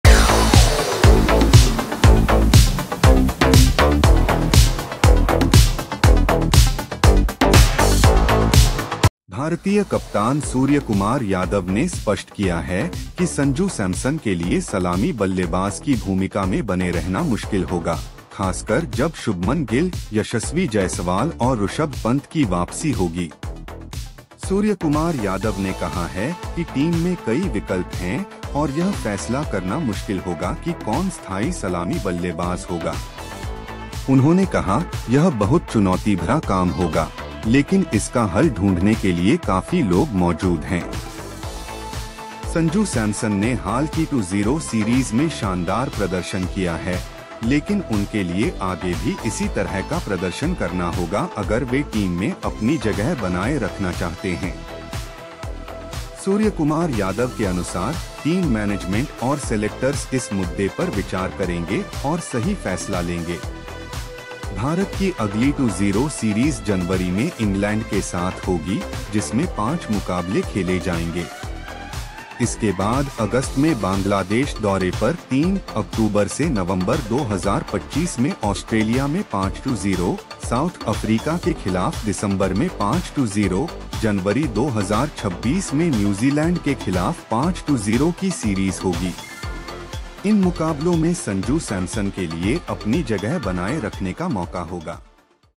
संजू सैमसन टी20 टीम में होंगे या नहीं? sanju samson
IND vs SA : Sanju has scored 3 centuries in last 5 innings!